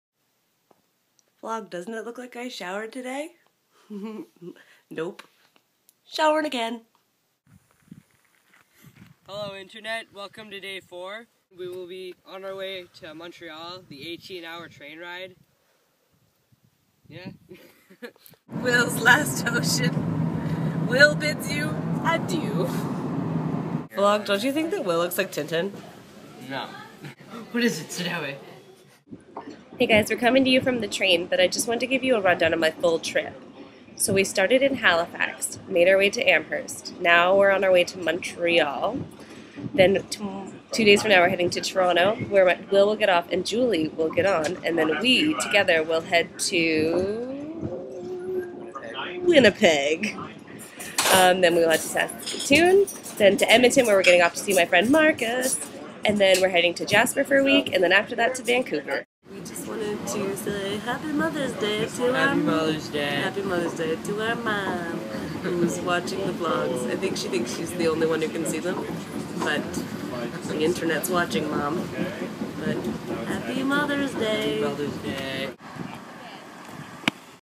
The Adventures of Tintin- Amherst/Train
The one where we start heading westward toward Montreal via an 18 hour VIA train ride. We shall getterdone!